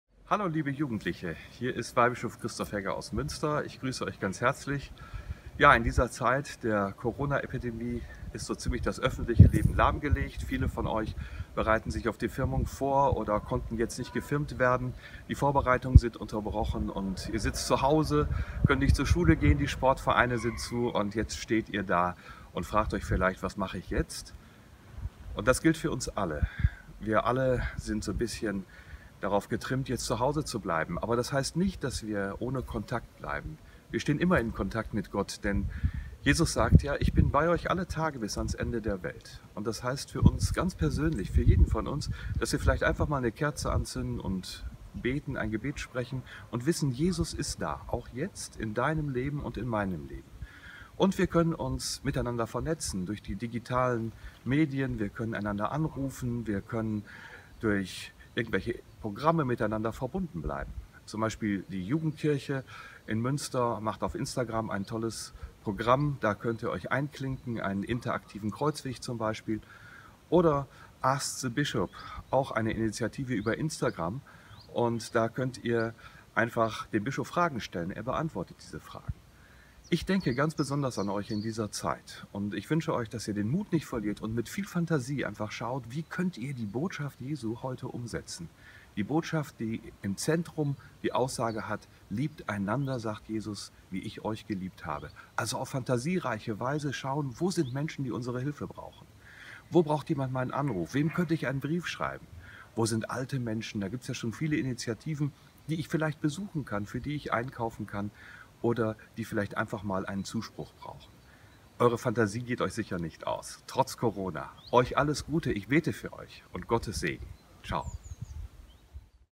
Videobotschaft von Weihbischof Hegge an die jungen Menschen im Bistum Münster in Zeiten von Corona.
In der Zeit der Corona-Pandemie wendet sich Weihbischof Dr. Christoph Hegge in einer Videobotschaft an die jungen Menschen im Bistum Münster.